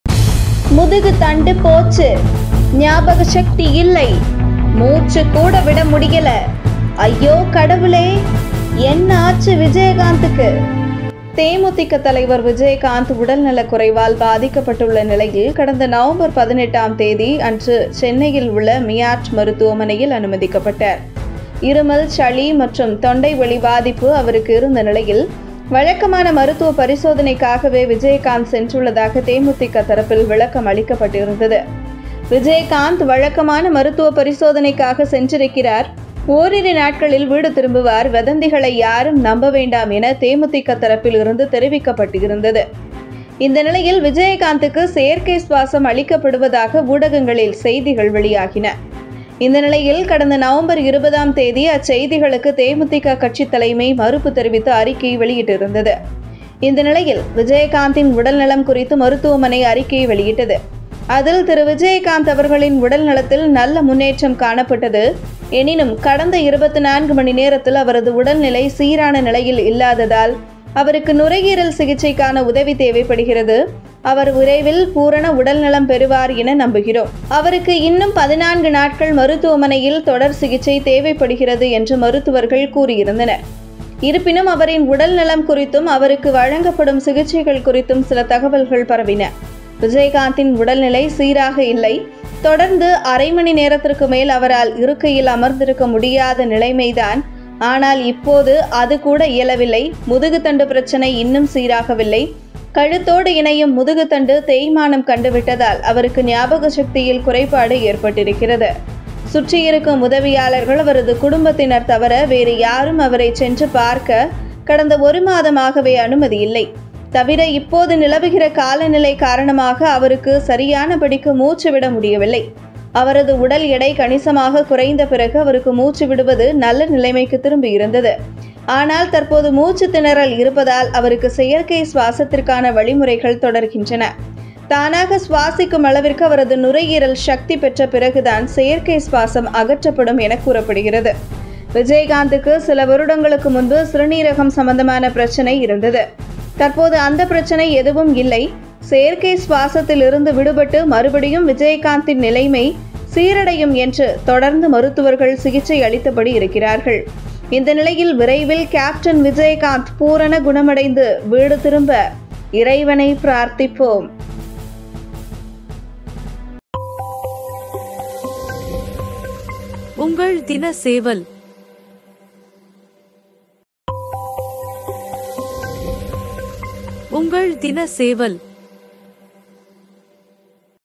முதுகு தண்டுபோச்சு..ஞாபக சக்திஇல்லை..மூச்சு கூடவிட முடியல..ஐயோ கடவுளே..என்னாச்சு விஜயகாந்த்துக்கு...
முதுகு தண்டு போச்சு... ஞாபக சக்தி இல்லை... மூச்சு கூட விட முடியல... ஐயோ கடவுளே... என்னாச்சு  விஜயகாந்த்துக்கு...